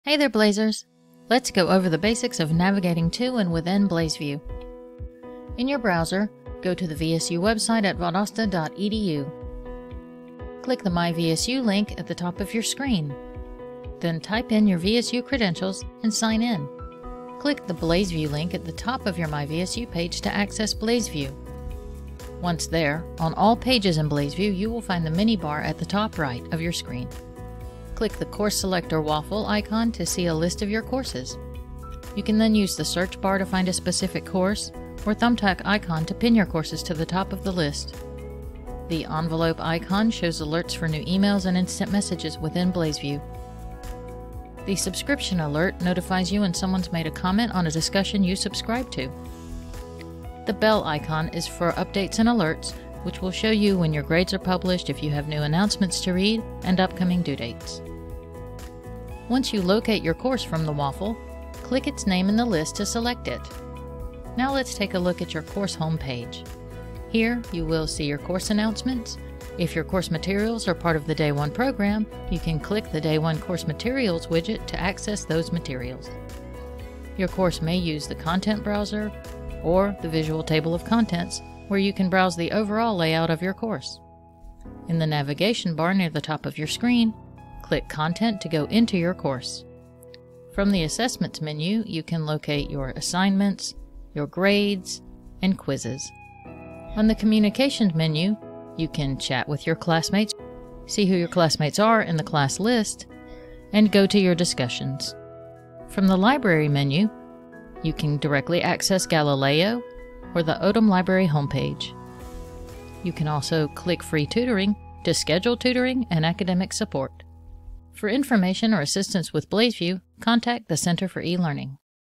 How to Navigate BlazeVIEW
Watch this video to review how to get to and navigate within BlazeVIEW.